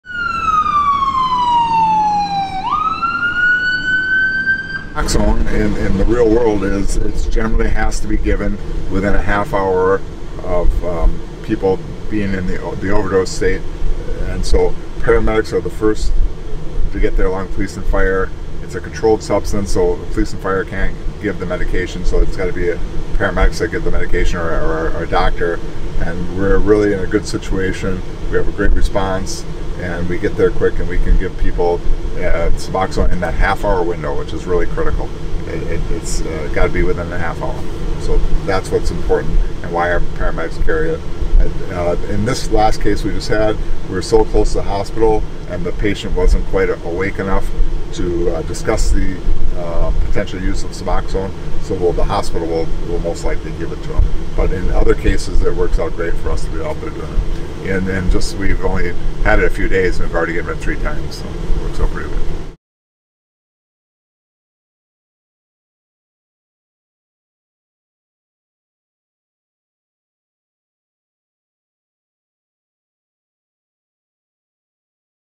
Hennepin EMS paramedics add new tool to save lives | Sahan Journal
Last week, Hennepin Healthcare Emergency Medical Services Assistant Chief Ryan Mayfield responded to an overdose call—a common enough scenario, especially on a summer Friday. Hennepin EMS paramedics treat about 10 to 20 overdoses a day.

In this case, bystanders told the 911 dispatcher that the man who had overdosed was not breathing, so they had started CPR. They also gave him naloxone, a drug that reverses opioid overdoses.